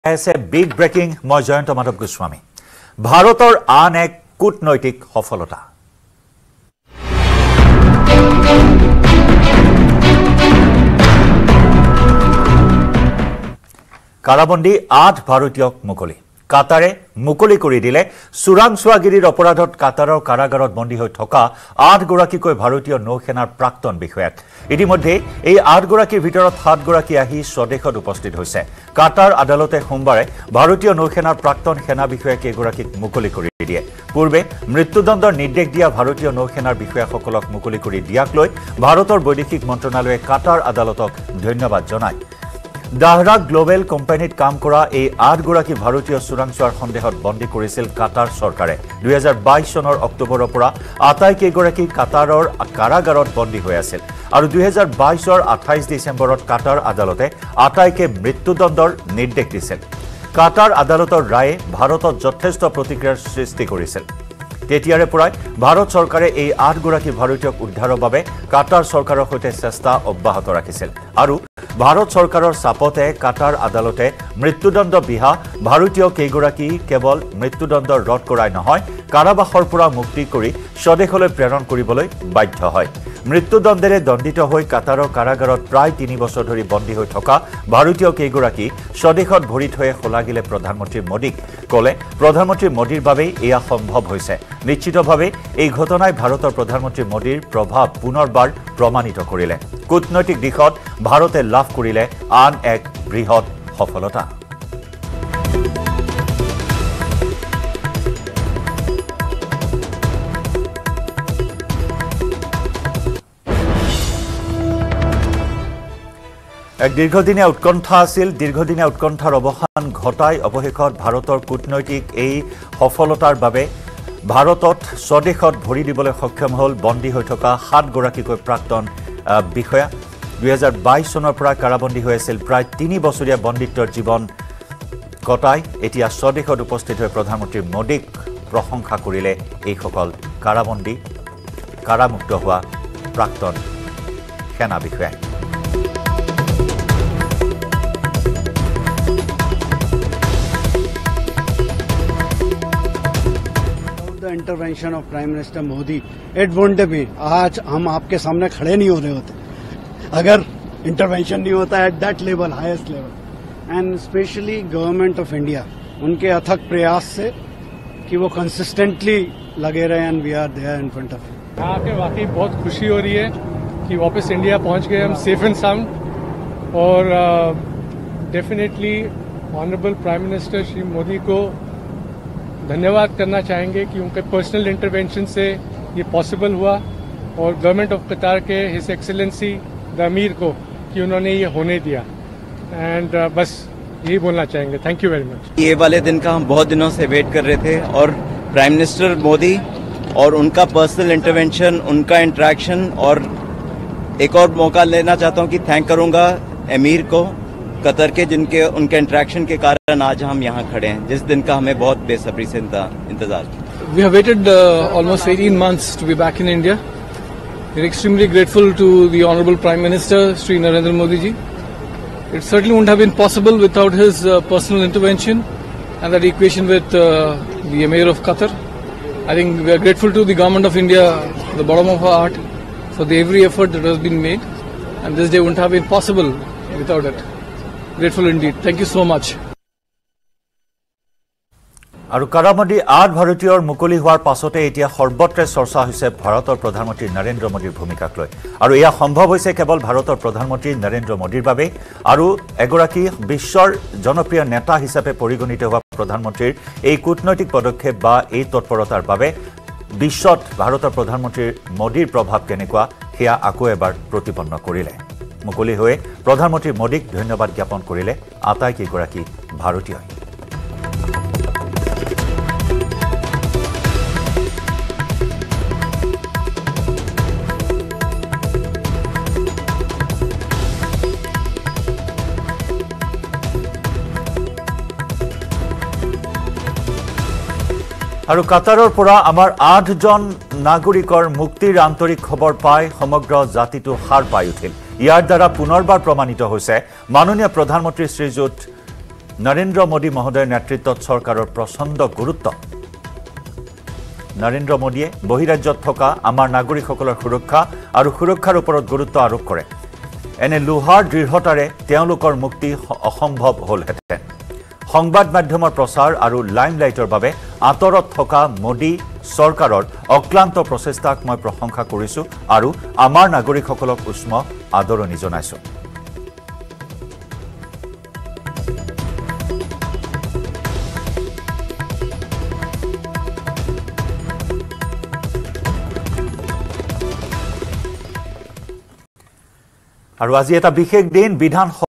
Qatar court releases 8 former Indian Navy personnel : ভাৰতৰ আন এক কূটনৈতিক সফলতা
Qatar court releases 8 former Indian Navy personnel | ভাৰতৰ আন এক কূটনৈতিক সফলতা। কাটাৰে মুকলি কৰি দিলে চোৰাংচোৱাগিৰিৰ অপৰাধত কাটাৰৰ কাৰাগাৰত বন্দী হৈ থকা ৮ গৰাকীকৈ ভাৰতীয় নৌ সেনাৰ প্ৰাক্তন বিষয়াক। ইতিমধ্যে এই আঠ গৰাকীৰ ভিতৰত সাত গৰাকী আহি স্বদেশত উপস্থিত হৈছেহি।